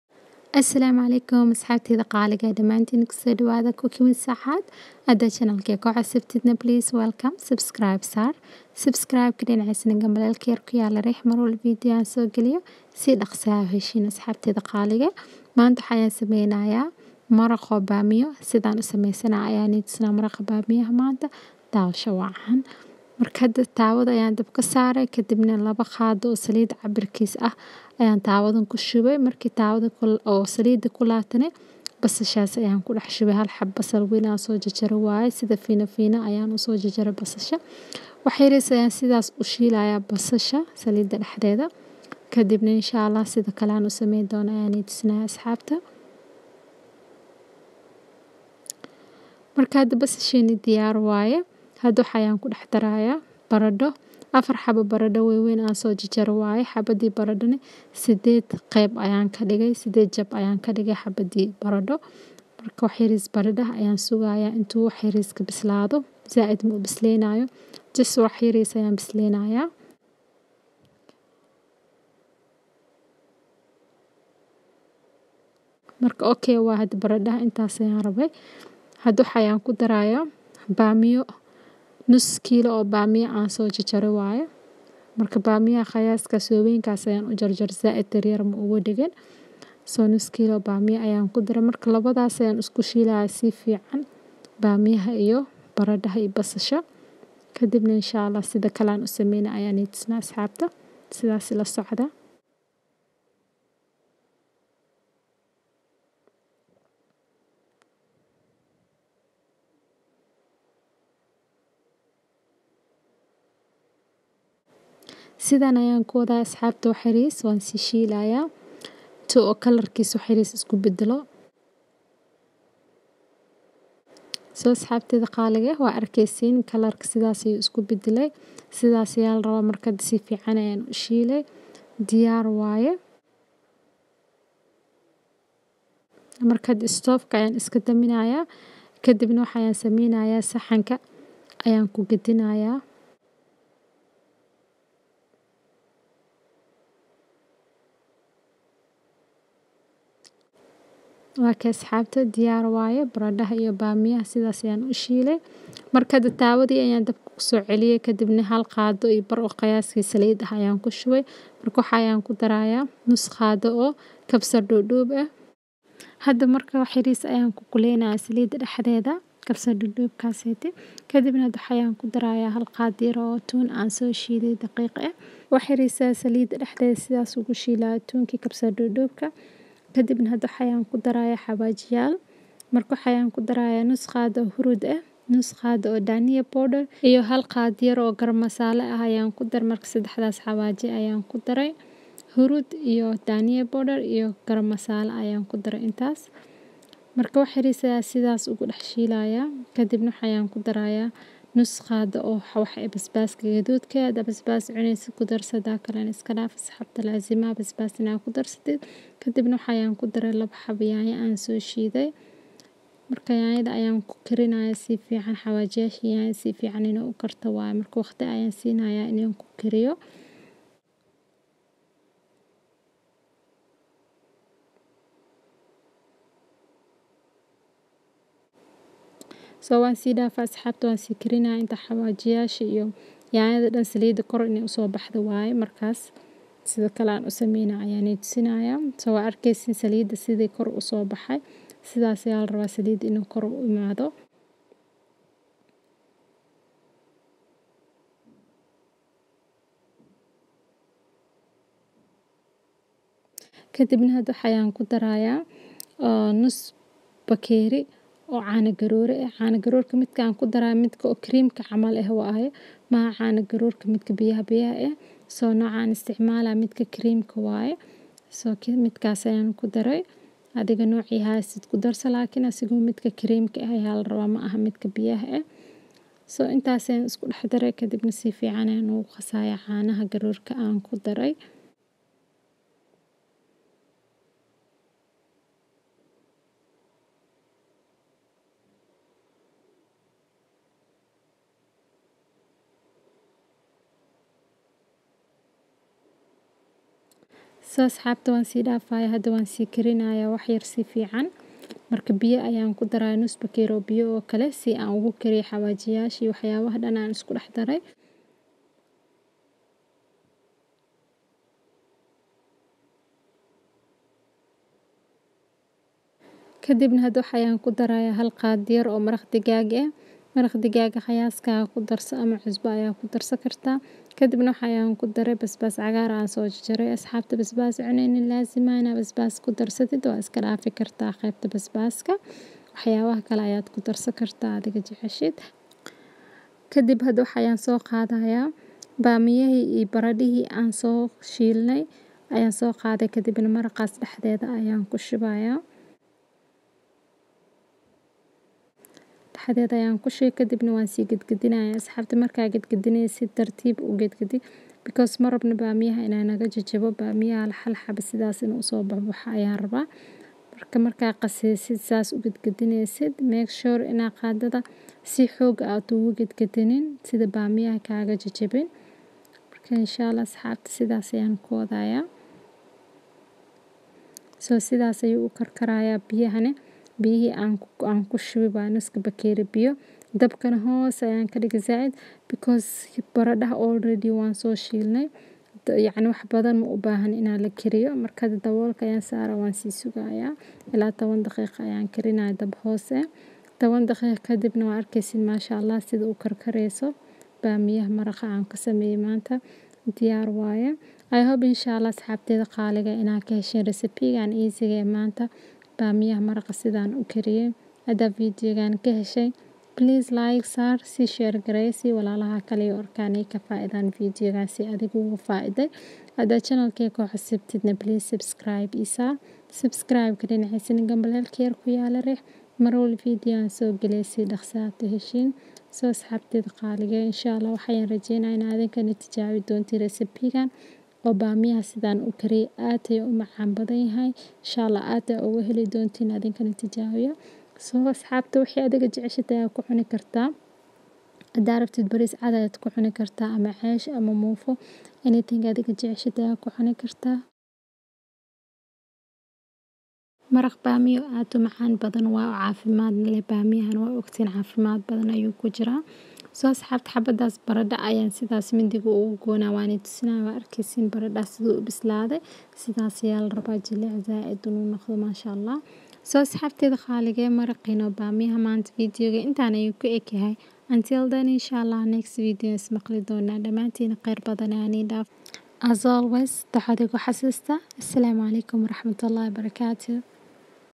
Maraaq bamiyo o caano garor laga sameyo | How To Make Masala Bhindi | Fried Masala Bhindi
How to make Bhindi aloo with Dahi recipe